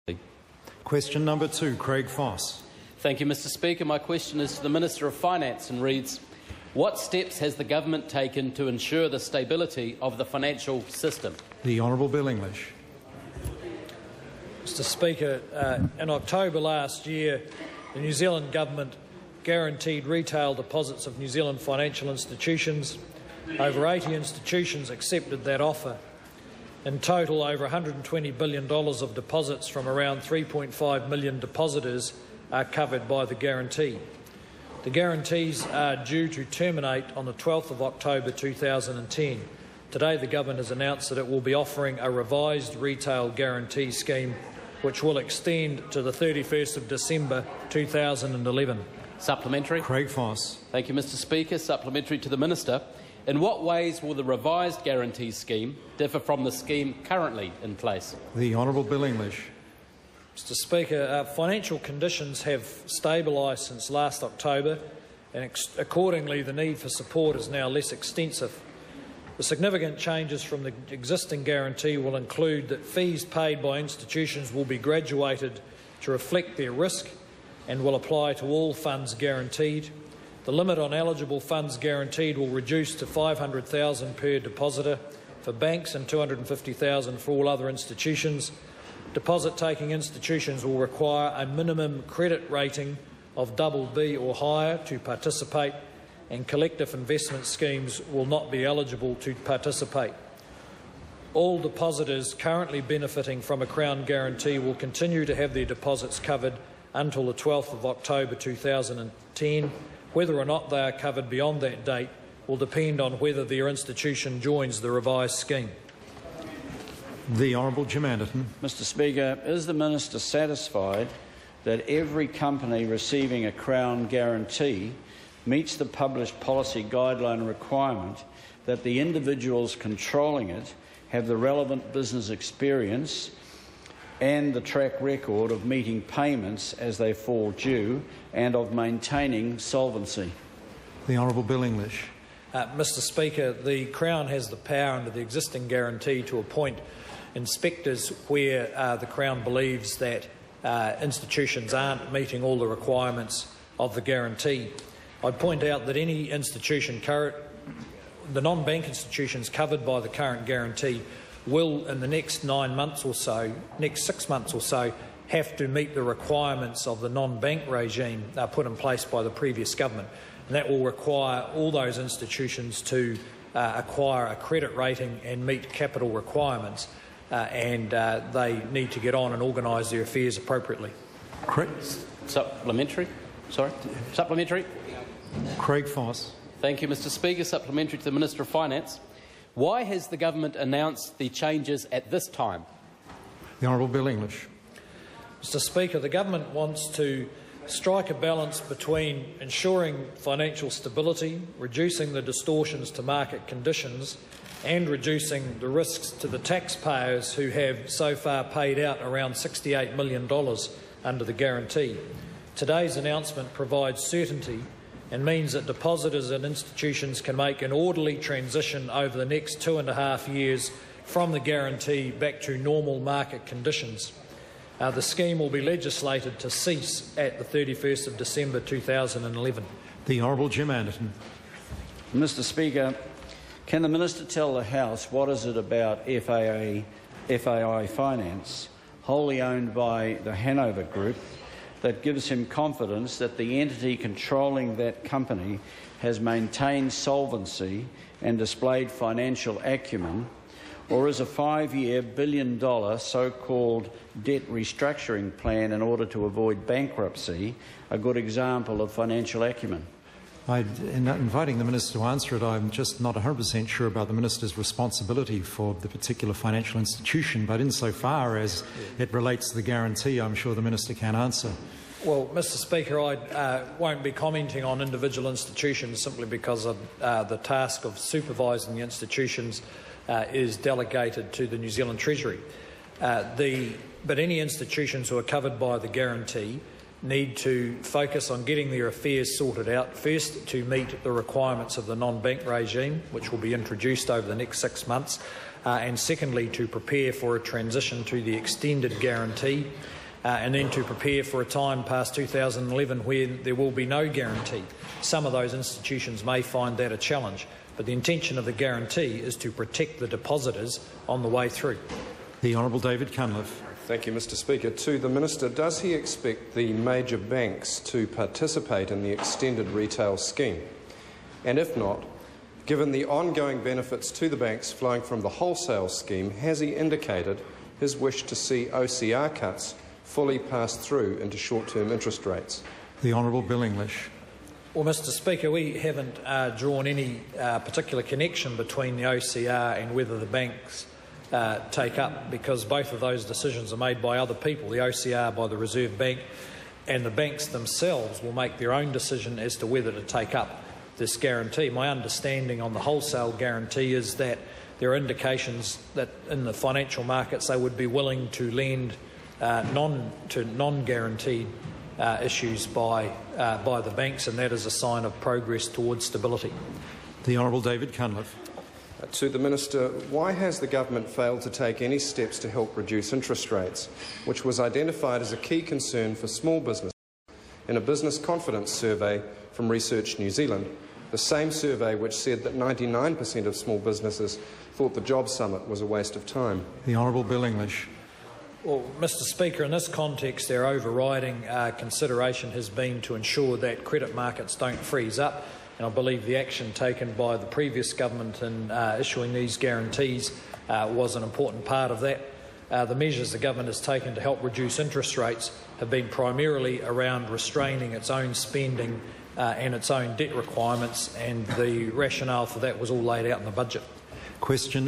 Aug25 questiontime q1
Bill English in question time, following the announcement to extend the retail deposit guarantee scheme.